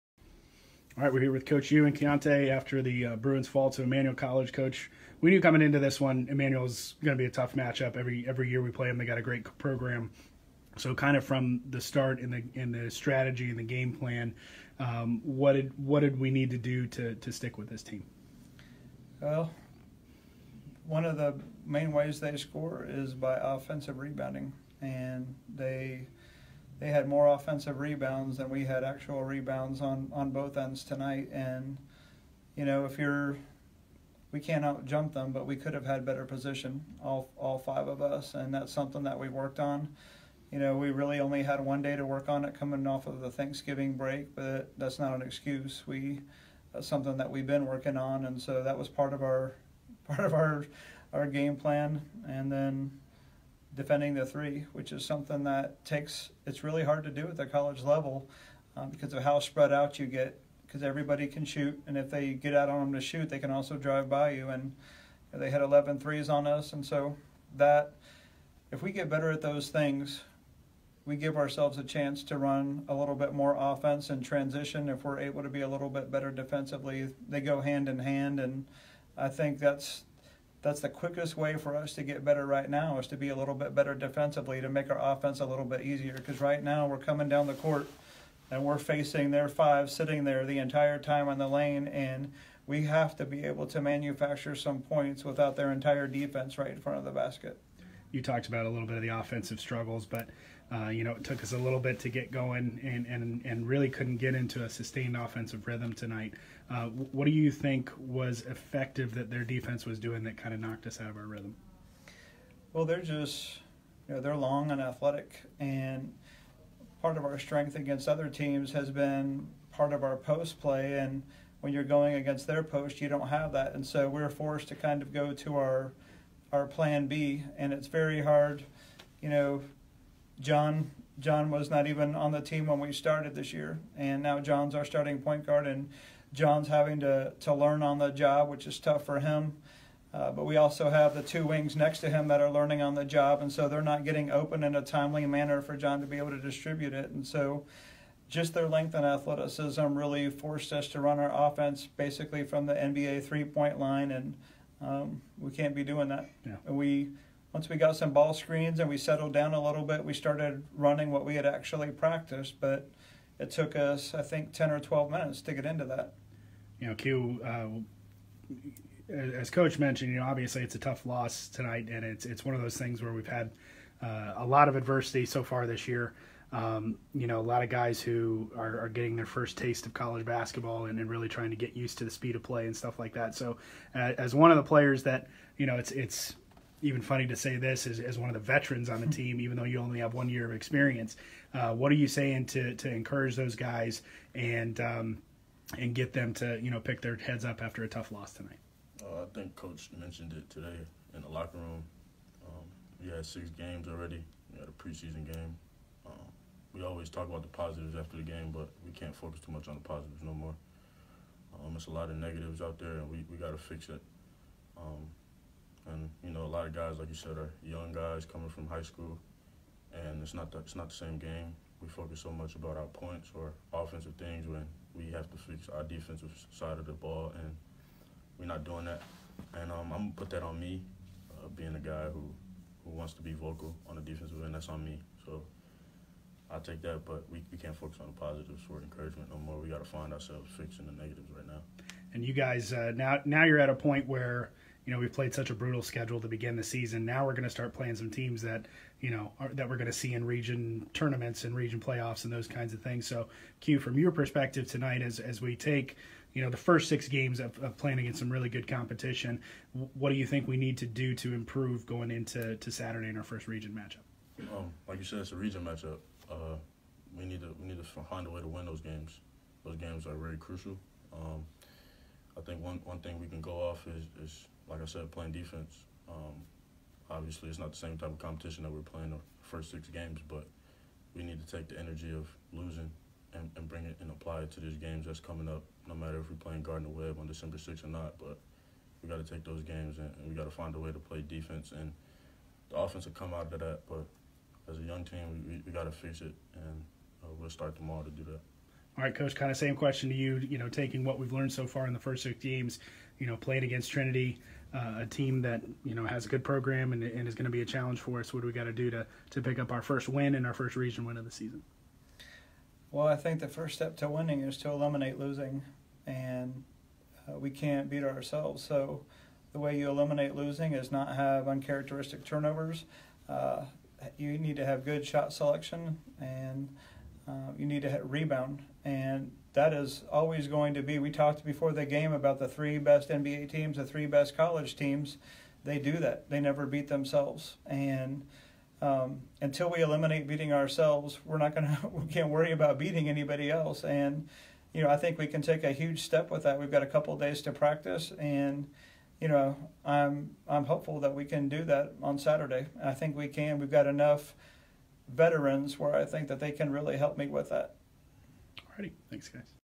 Postgame Press Conference - Men's Basketball - 11/27/18
Coach Uwarow and Quiyante Burroughs talk about the Bruins loss to Emmanuel College.